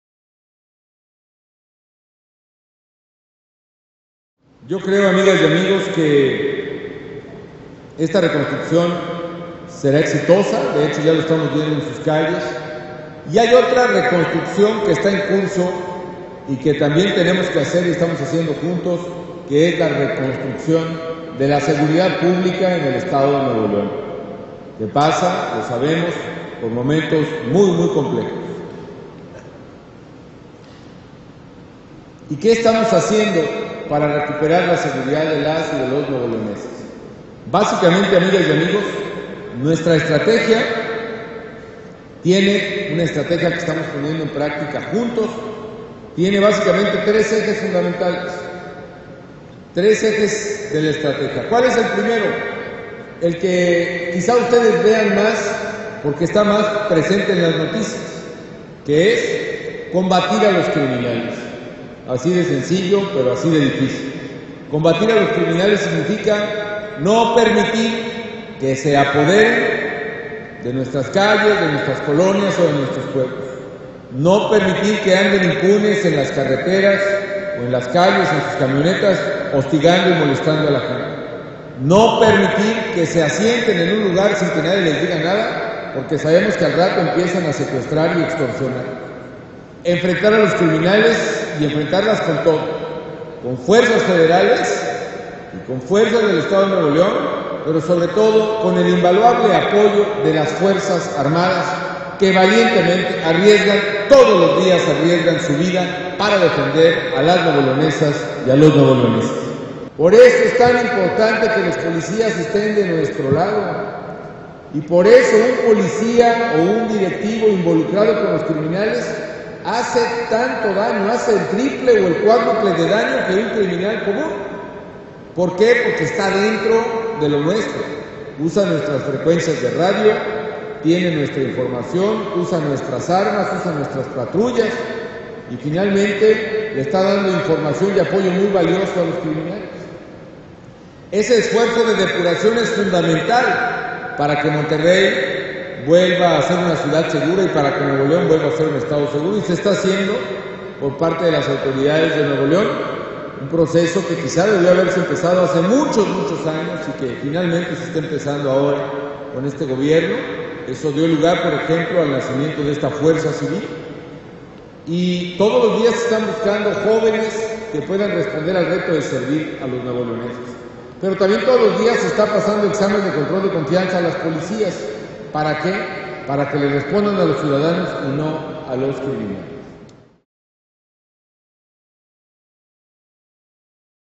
Exige Calderón acabar con corrupción
De visita en NL, Calderón dijo que trabajan en reconstruir y fortalecer las instituciones de seguridad a través de la depuración de los cuerpos policiacos.